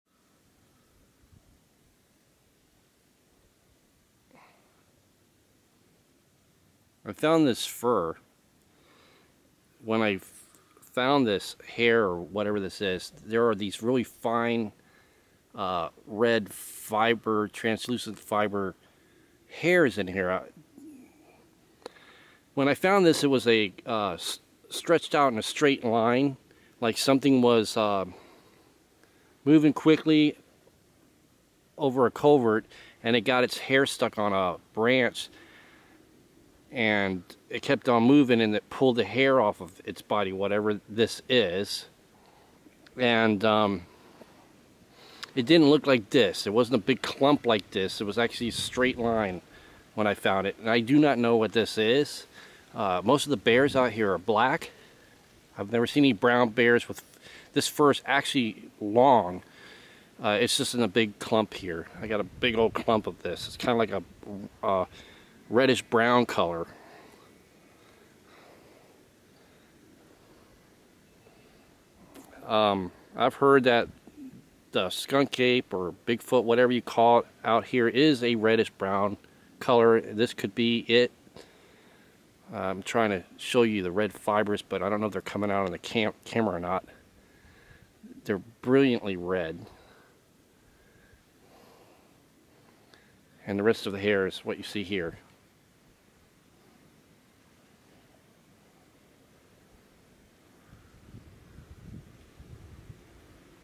Vid # 75 Bigfoot hair or something else ?
Hair was located on a branch  near a covert in FL swamp probably was moving fast because hair was laid out in straight line from branch toward ground I never seen bears or cats with this color hair in area.Under magnify glass didnt need microscope the hair has 3 colors in it Brown ,Black & those bright Red ones . 2/19/20